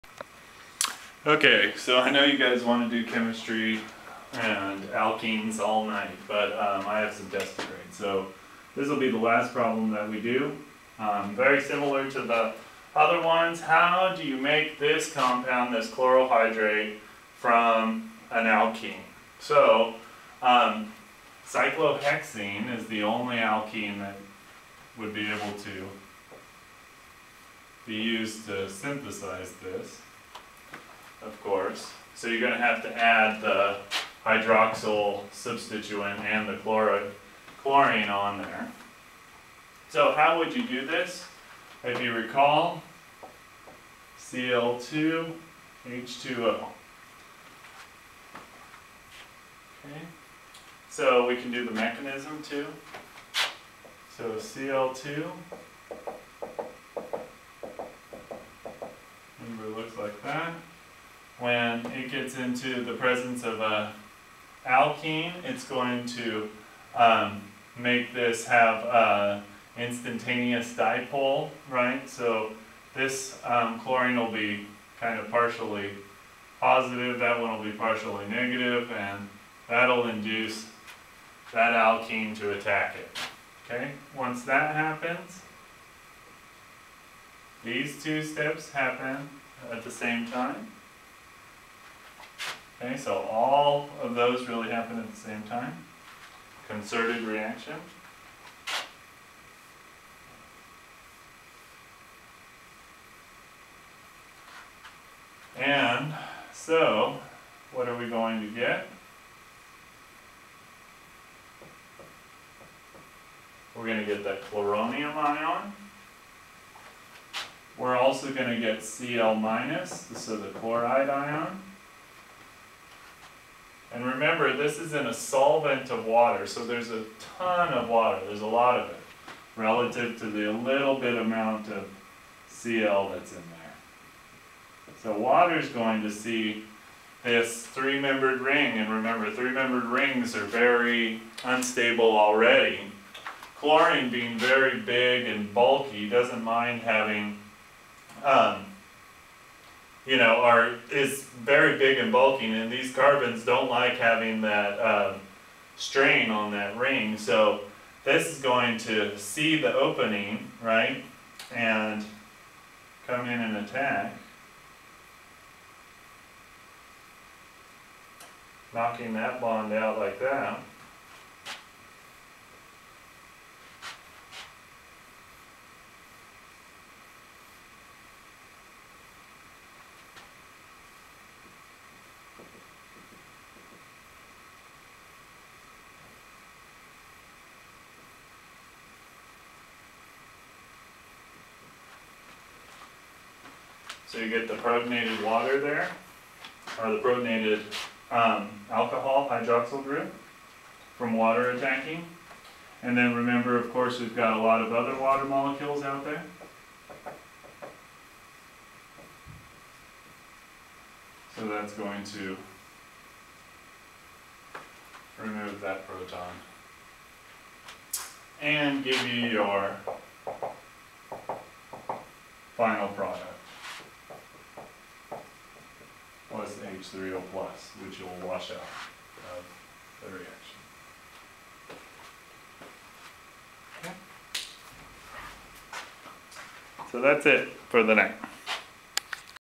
Halohydrin Formation - Addition of Cl2/H2O to an Alkene 001
Indicate how to form the pictured chlorohydrate from your choice of alkene. Draw the mechanism of the reaction.

Interviews
1) Revell, K. (November 16, 2016) “An Interview with Heath Giesbrecht, Part I” The Macmillan Community

2) Revell, K. (November 16, 2016) “An Interview with Heath Giesbrecht, Part II” The Macmillan Community